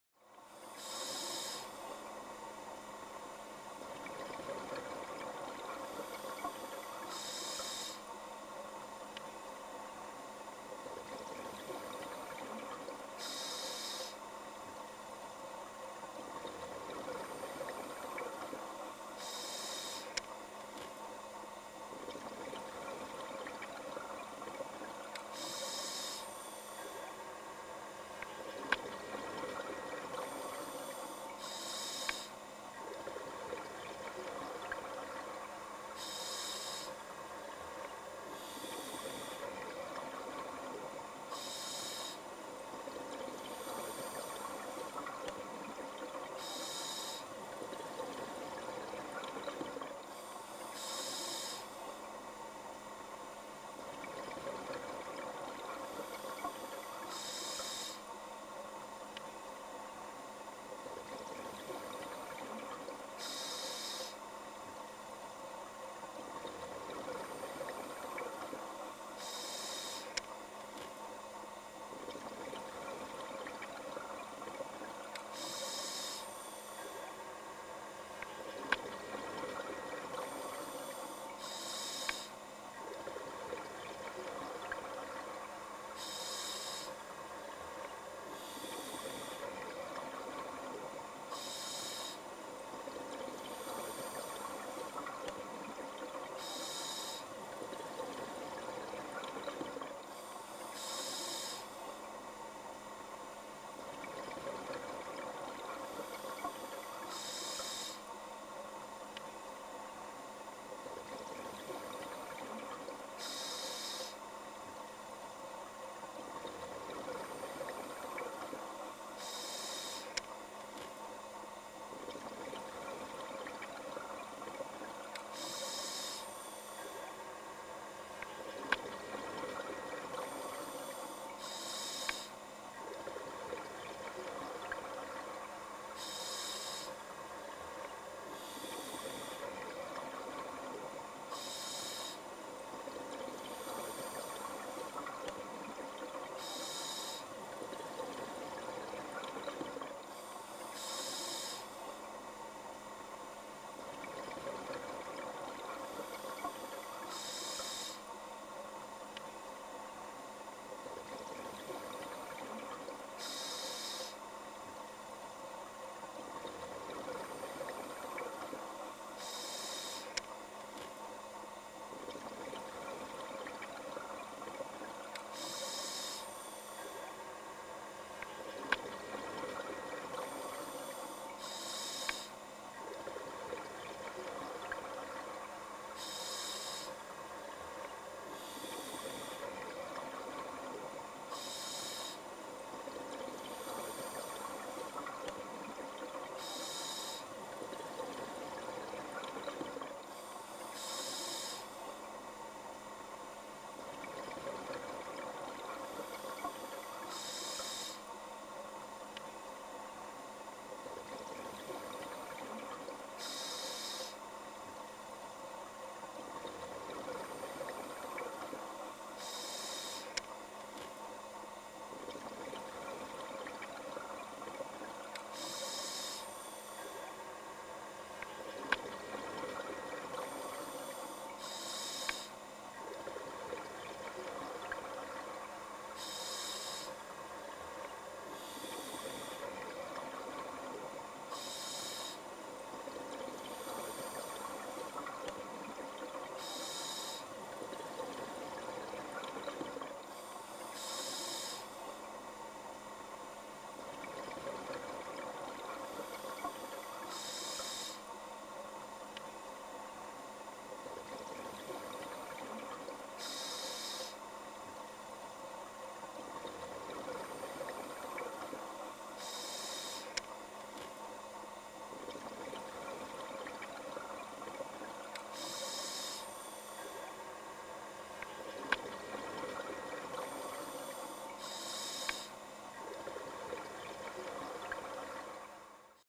Dead man's thumb that with fine polyps food filters (soft corals) 🔴
Content by: Nudibranch MDP (producent);Peter van Rodijnen (camera);Stichting Natuurbeelden

Description (more): Orange and white copies dead man's thumb, where the filtering operation of the polyps is clearly visible.